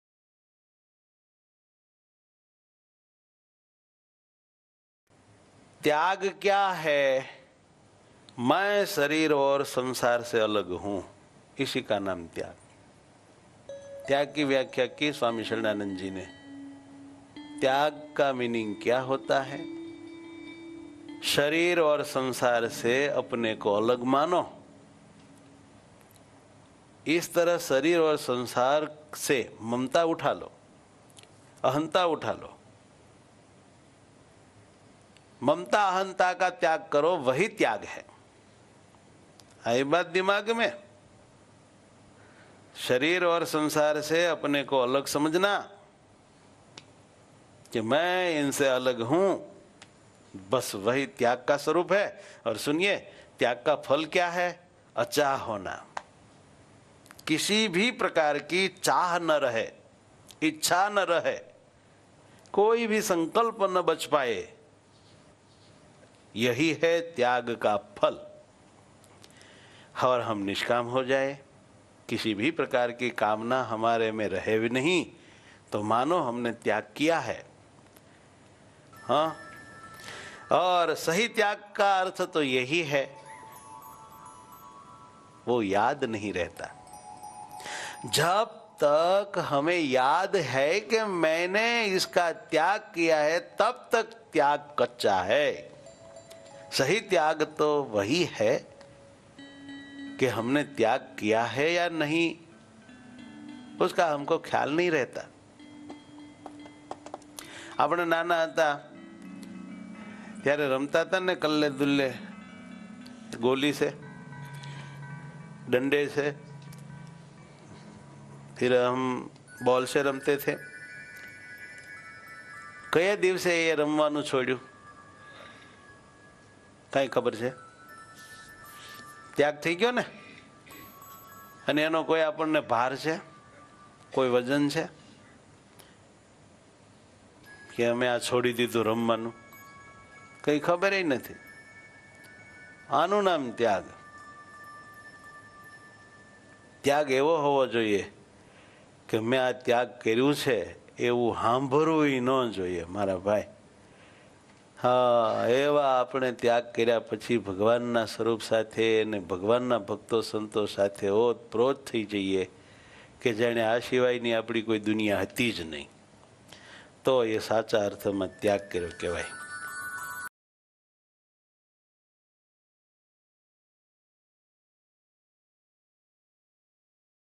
Gyandhara | જ્ઞાનધારા | Sacho Tyag | 24 Jul 2020 | Pu. Gyanjivandasji Swami - Kundaldham
Title: Sacho Tyag
Guj. Title: સાચો ત્યાગ
Type: Katha
Category: Gyandhara
Orator: Pu. Gyanjivandasji Swami - Kundaldham
Event Date: 24 Jul 2020

Playlist Name: Gyandhara | જ્ઞાનધારા | By Pu.Gyanjivandasji Swami - Kundaldham